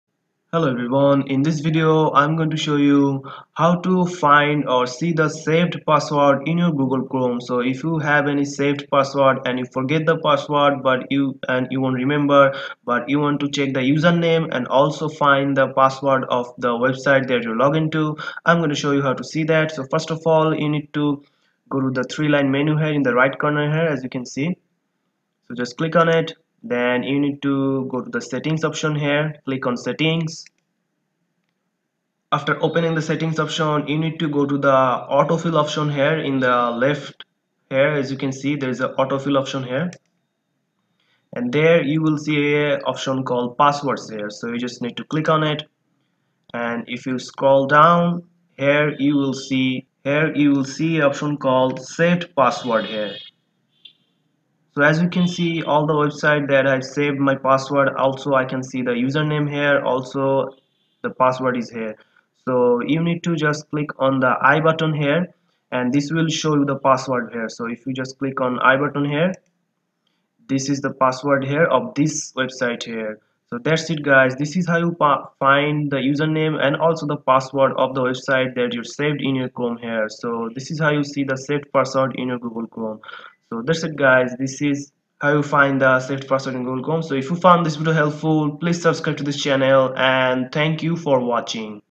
How to Find & Recover Saved Passwords in Google Chrome | see passwords saved in chrome 2022
In this video, we show you how to find and recover your saved username and passwords in Google Chrome. Google Chrome  is allowing you to save your website username and passwords in the browser.
so if you forgot your password or username you can  recover and find password and username from chrome setting .
its easy to see Passwords saved in chrome.

00:00 intro
00:20 How to Find & Recover Saved Passwords in Google Chrome